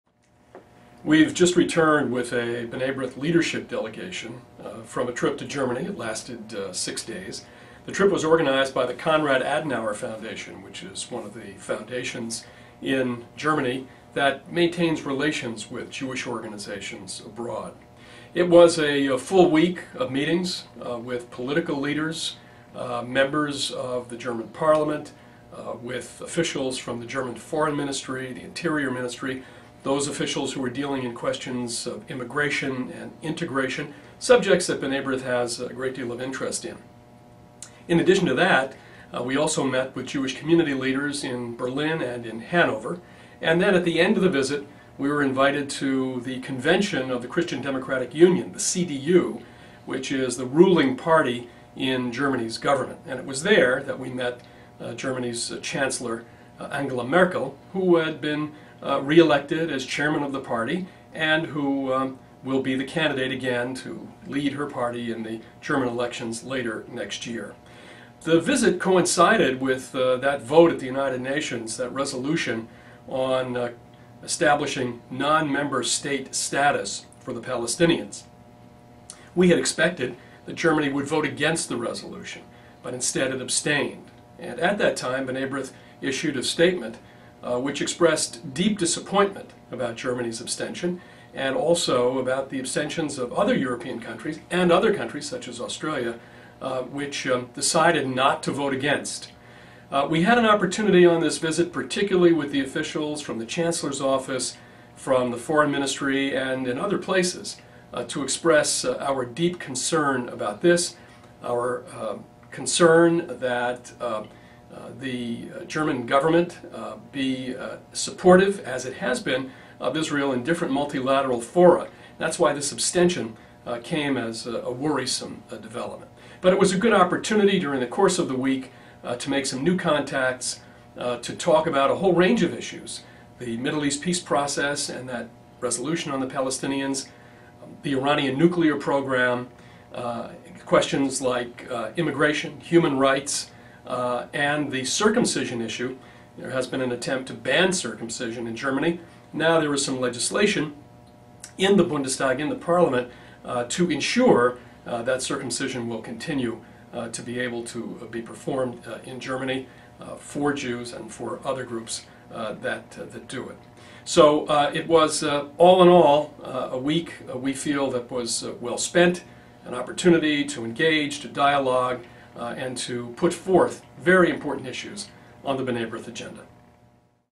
B'nai B'rith Policy Update: Leadership Mission to Germany Recap
Daniel S. Mariashin, executive vice president of B'nai B'rith International, discusses this past week's delegation of B'nai Brith leaders, led by International President Allan J. Jacobs, to Germany.  The trip was organized by the Konrad Adenauer Foundation.

The group met with leading members of the Bundestag, senior government officials representing the ministries of foreign affairs and interior, representatives of government agencies responsible for migration and integration and senior advisers in the chancellor's office. U.S. Ambassador Phillip Murphy hosted the delegation at the American Embassy in Berlin. Topics of discussion included the Middle East peace process, Iran's nuclear program, transatlantic relations and recent attempts in Germany to ban circumcision.

B'nai B'rith paid a visit to the Central Council of Jews in Germany and its director, Stephan Kramer. They also visited the Memorial for the Murdered Jews in Europe and other Jewish sites in Berlin. The delegation met with leaders of the Jewish community, as well as sites of Jewish interest in Hannover.

The delegation concluded its visit in Hannover as guests of the Christian Democratic Union (CDU), Germany's ruling party, at its convention. The convention re-elected Angela Merkel as party leader, and nominated her to head the CDU list in next year's parliamentary elections. The group met Merkel during the party gathering, two days before her meeting with Israeli Prime Minister Benjamin Netanyahu, in Berlin.

The B'nai B'rith International mission took place only days after the vote in the U.N. General Assembly on a resolution granting non-member state status to the Palestinians. Germany, which had been expected to vote against the resolution, abstained instead. B'nai B'rith, in its statement on the vote, expressed "deep disappointment" in Germany's abstention and those of other European countries, such as the United Kingdom.

Before 1933, B'nai B'rith had more than 100 lodges throughout Germany. Re-established in the mid-1960s, there are today active lodges in several German cities.

In addition to Jacobs and Mariaschin, the delegation included: Ira Bartfield, B'nai B'rith International senior vice president; Irving Silver, Vice-Chairman of B'nai B'rith International's Center for Human Rights and Public Policy (ICHRPP); Abraham Mitchell, member of the Board of Governors; Eduard Redensky, of the B'nai B'rith Young Professionals Network; Michael Gellman, ICHRPP board member; Ralph Hofmann, president of B'nai B'rith Europe and Eric Fusfield, ICHRPP deputy director.